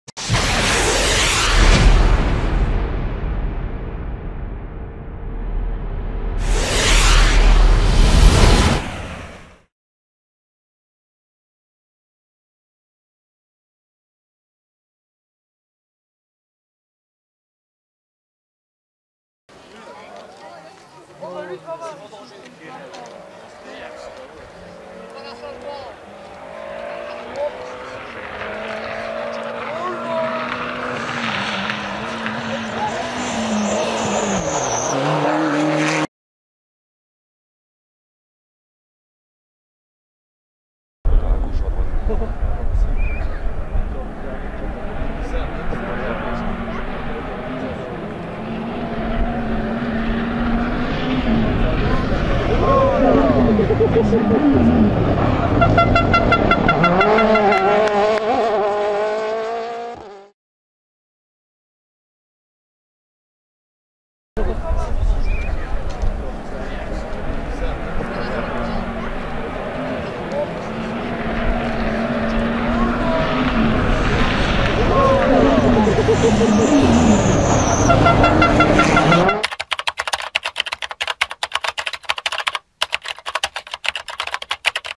RALLYE DU CONDROZ PATRICK SNIJERS 1990 ET 2012 ES WANZOUL
Comparatif sur la ES de Wanzoul en 1990 et 2012